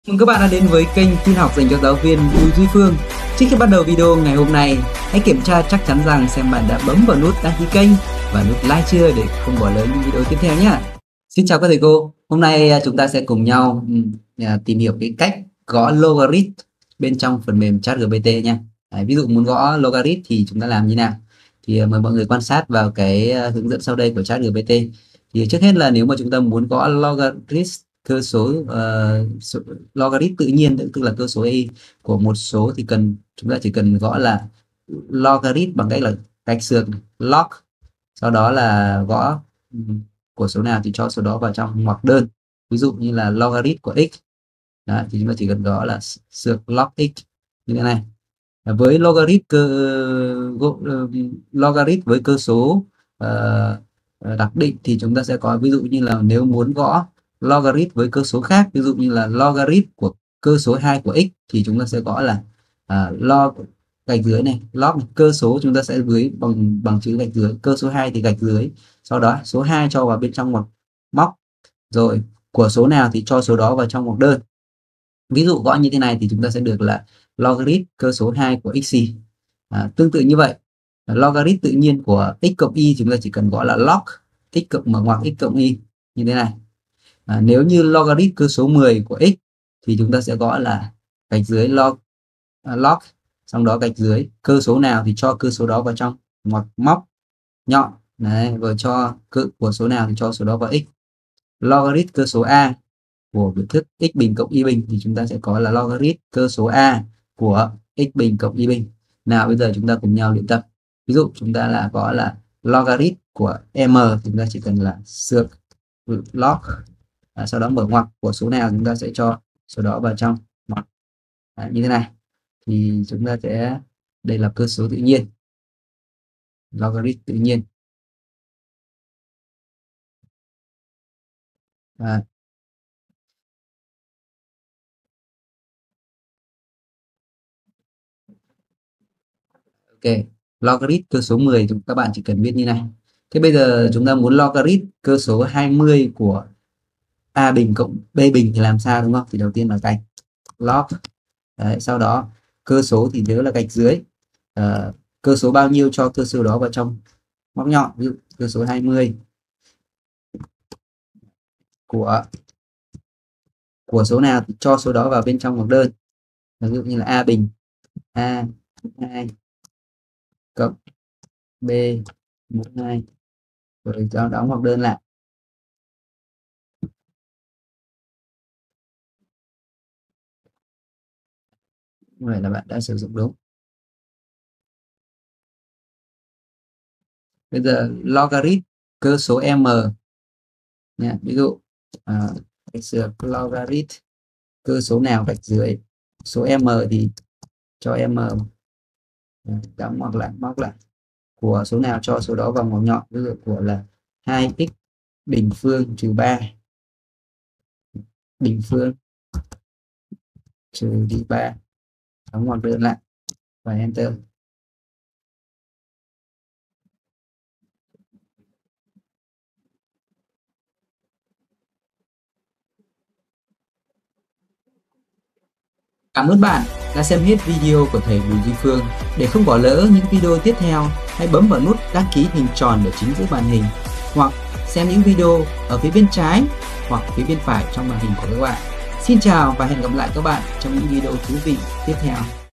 Cách gõ Logarit trong Chat GPT| Bùi Duy Phương
Logarit là một khái niệm toán học quan trọng và có nhiều ứng dụng trong các lĩnh vực như khoa học, kỹ thuật, và thống kê. Việc hiểu và sử dụng Logarit một cách chính xác có thể giúp giải quyết nhiều bài toán phức tạp một cách hiệu quả.

Trong video này, chúng tôi sẽ hướng dẫn bạn cách gõ biểu thức Logarit một cách dễ dàng và linh hoạt trong cuộc trò chuyện với GPT. Chúng ta sẽ tìm hiểu cách sử dụng các biểu tượng và cú pháp phù hợp để biểu diễn Logarit một cách chính xác và rõ ràng.

Dù bạn là người mới bắt đầu hay đã có kinh nghiệm trong việc sử dụng Logarit, video này sẽ cung cấp cho bạn những mẹo và kỹ thuật để tương tác với GPT một cách hiệu quả và thuận tiện.

Người hướng dẫn:
►Teacher: Bùi Duy Phương